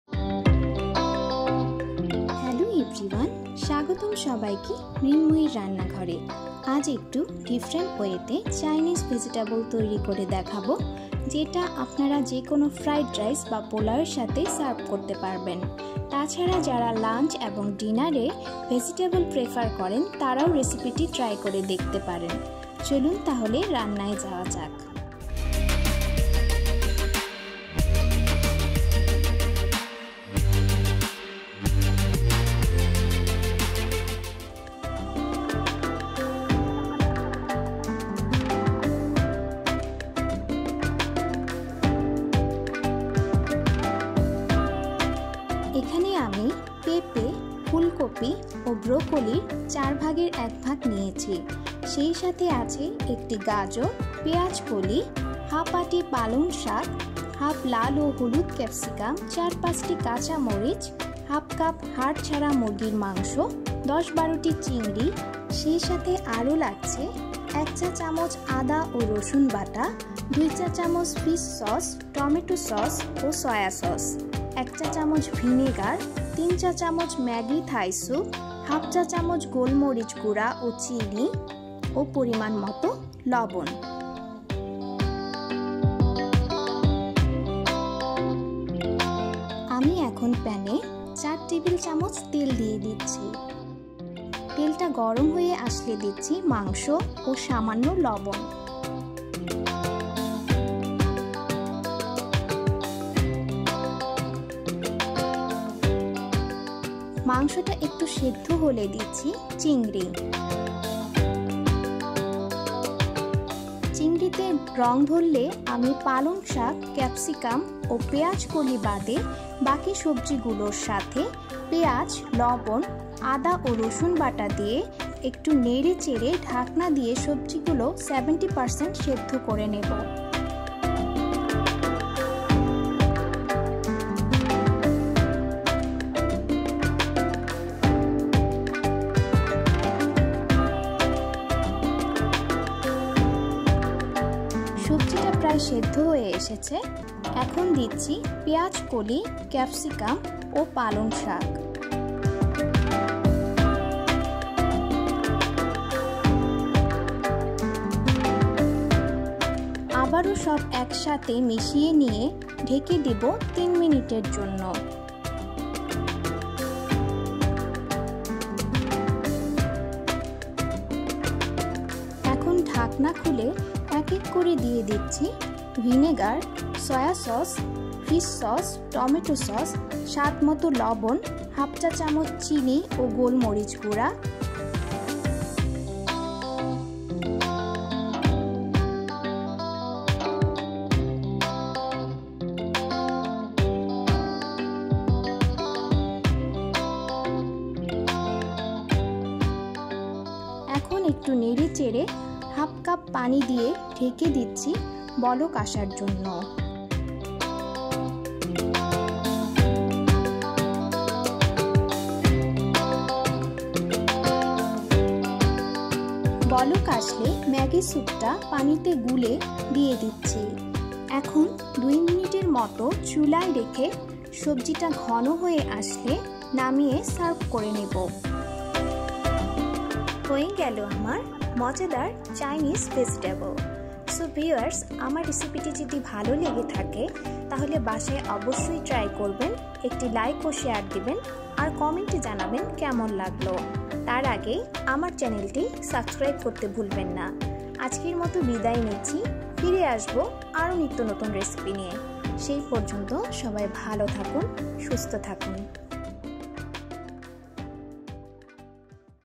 Chinese Vegetable | রেস্টুরেন্টের স্বাদকে হার মানানো চাইনিজ ভেজিটেবল | Mixed Vegetable
Personally, this is my favorite Chinese dish. Lots of fun to eat and quite healthy. Chinese vegetable to cook but does not take too much time as well as a very simple recipe. Learn how to make Chinese Vegetable/চাইনিজ ভেজিটেবল recipe.To make this recipe needs few numbers of ingredients. So you must try this Chinese Vegetable Recipe | Mixed Vegetable recipe at home and enjoy.

Ingredients that look like:

1. 1/2 cup chicken

2. Shrimp

3. Papaya
4. Cauliflower

5. Broccoli

6. Carrots
7 . Onion sprouts

8. Spinach
9. Capsicum
10. Pepper

11. 1 tsp of ginger paste
12. 1 tsp of garlic paste
13. 2 tsp of fish sauce
14. 2 tsp of tomato sauce
15. 2 tsp of soya sauce
16. 1 tsp of vinegar
17. 3 tsp of Maggi Thai soup

18. ½ tsp of white piper powder
19. 1 teaspoon sugar
20. Salt

21. Oil

উপকরণ যা লাগছে :

১. ১/২ কাপ মুরগির মাংস
২. চিংড়ি মাছ
৩. পেঁপে
৪. ফুলকফি
৫. ব্রকলি
৬. গাজর
৭. পেঁয়াজ কলি
৮. পালং শাক
৯. ক্যাপসিকাম
১০. কাঁচামরিচ
১১. ১ চামচ  আদা বাটা
১২. ১ চামচ রসুন বাটা
১৩. ২ চামচ ফিস সস
১৪. ২ চামচ টমেটো সস
১৫. ২ চামচ সয়া সস
১৬. ১ চামচ ভিনেগার
১৭. ৩ চামচ ম্যাগি থাই স্যুপ

১৮. ঙ্গ চামচ সাদা গোল মরিচ গুড়া
১৯. ১ চামচ চিনি
২০. লবন
২১. তেল

Regards
Mrinmoyeer Rannaghor